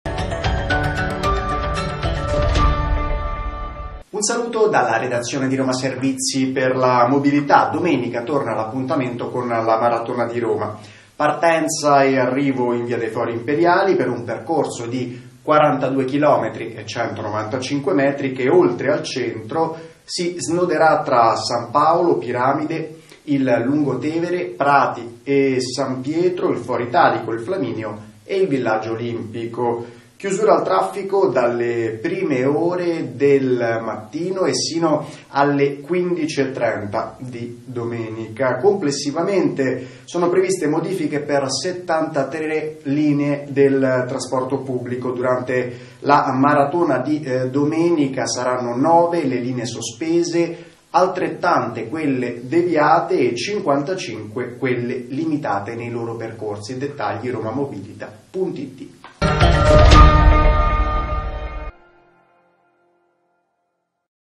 Tg della mobilità, domenica la maratona di Roma
Domenica torna l’appuntamento con la maratona di Roma. Partenza e arrivo in via dei Fori Imperiali per un percorso di 42 km che oltre al Centro si snoderà tra San Paolo, Piramide, il lungotevere, Prati e San Pietro, il Foro Italico, il Flaminio e il Villaggio Olimpico. Chiusure al traffico dalle prime ore e sino alle 15,30 di domenica. Complessivamente previste modifiche per 73 linee del trasporto pubblico: durante la gara 9 saranno sospese, 9 deviate e 55 limitate.